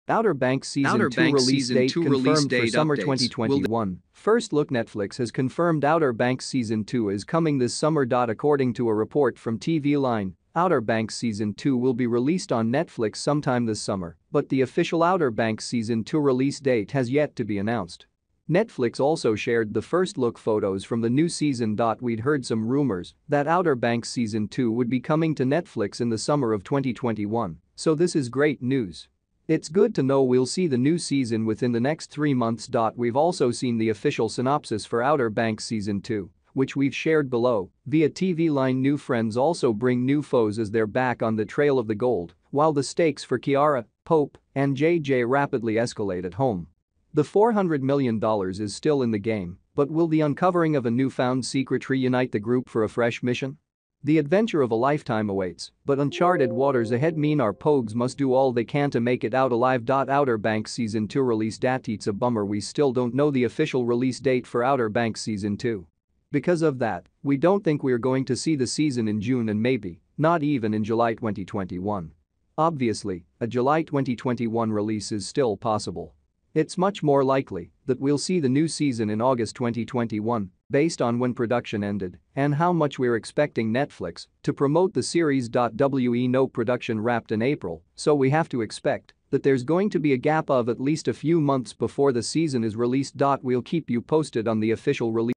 Outer Banks season 2 release date confirmed
Outer Banks season 2 release date confirmed for summer 2021: First look
Netflix has confirmed Outer Banks season 2 is coming this summer.

According to a report from TV Line, Outer Banks season 2 will be released on Netflix sometime this summer, but the official Outer Banks season 2 release date has yet to be announced. Netflix also shared the first look photos from the new season.

We’d heard some rumors that Outer Banks season 2 would be coming to Netflix in the summer of 2021, so this is great news! It’s good to know we’ll see the new season within the next three months.

We’ve also seen the official synopsis for Outer Banks season 2, which we’ve shared below, via TV Line: